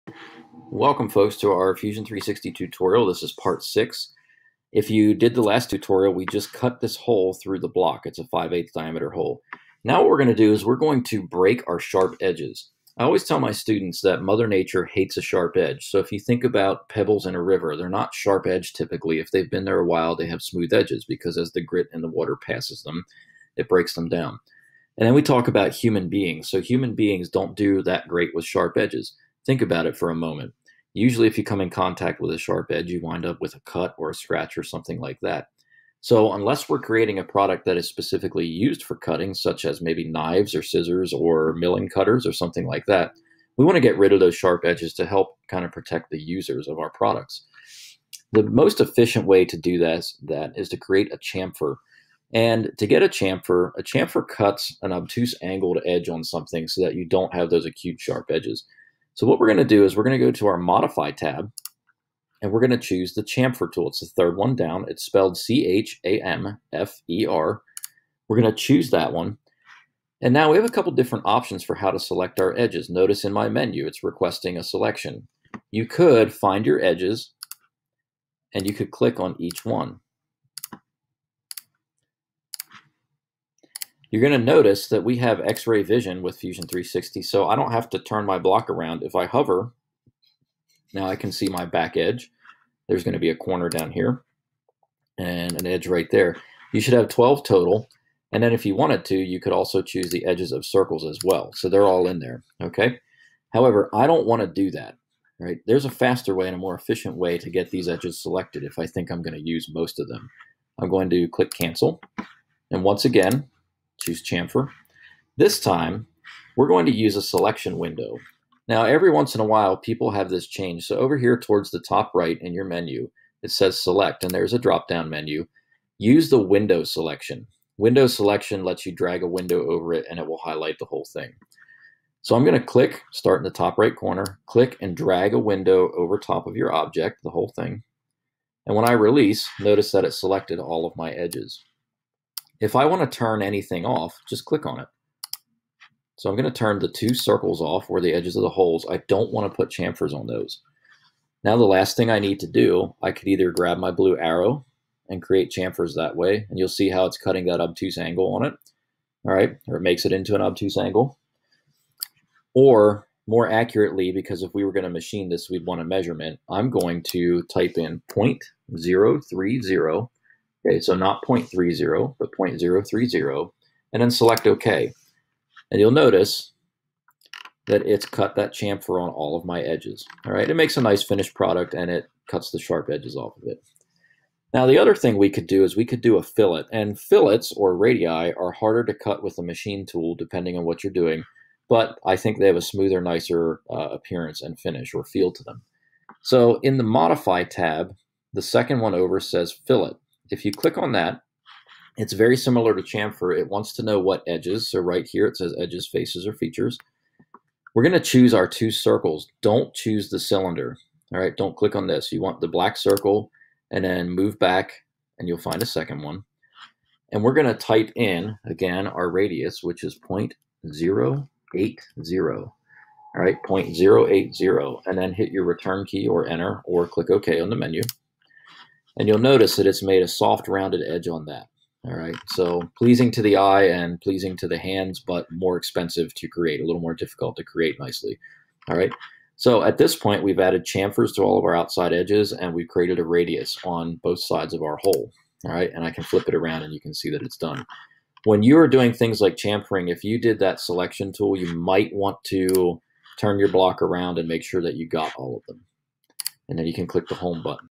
Fusion 360 Part 6 (Chamfers / Fillets)
In this tutorial, we learn how to "break edges" by adding chamfer and fillets/radii.  We also learn how to select and modify individual edges, or how to select all of the edges at once.